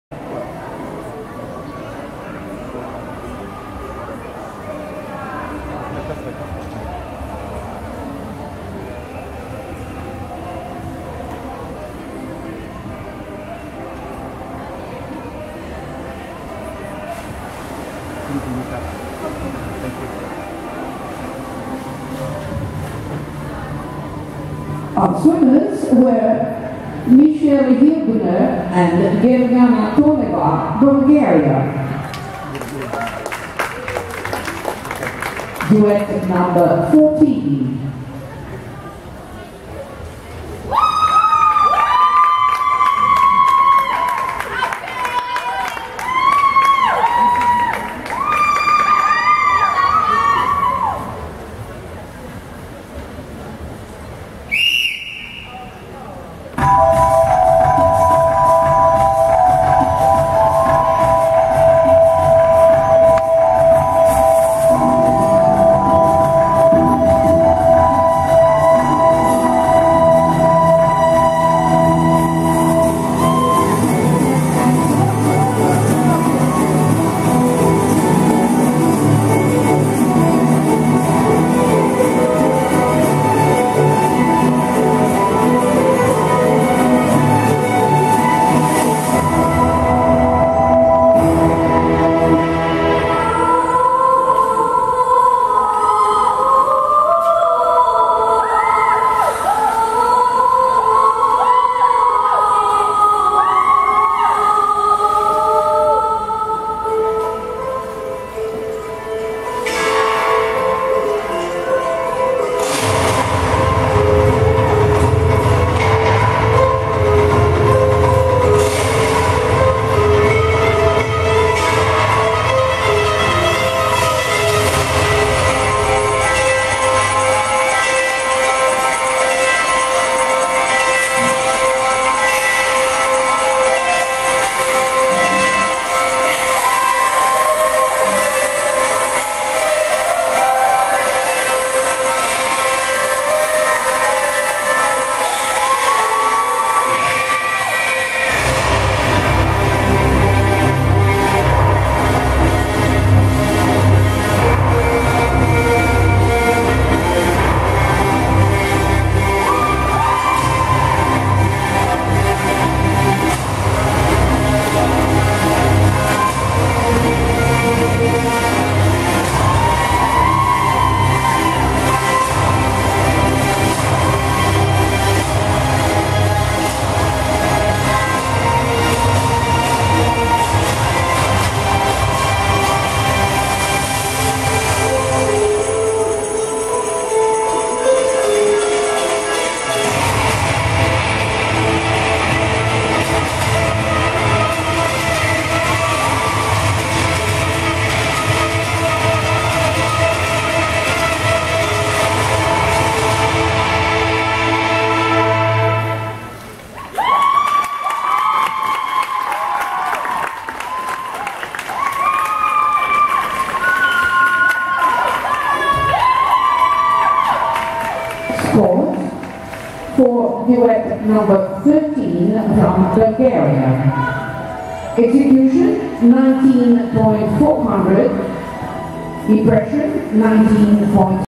German Open 2015 Synchronschwimmen
Technisches Duett Marlene Bojer und Justine Seibert